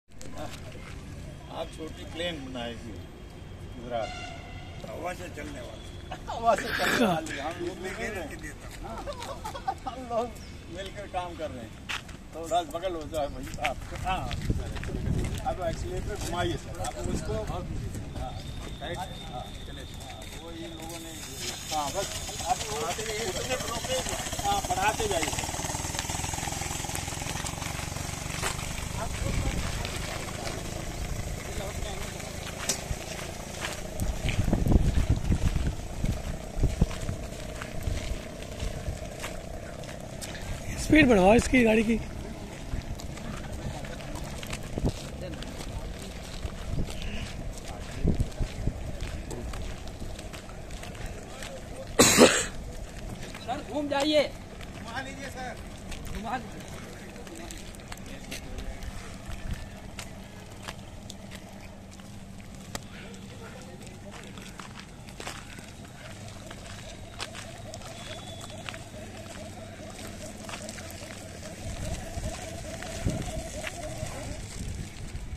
Air-O-Bike-Run by Dhanji Bhai Patel (23-09-2020) at 12:30 PM
Dhanji Bhai Patel an Industrialist and MLA of Gujarat State Govt. visited SMS and took feel of Running Air-O-Bike. He also  discuss its development for public use.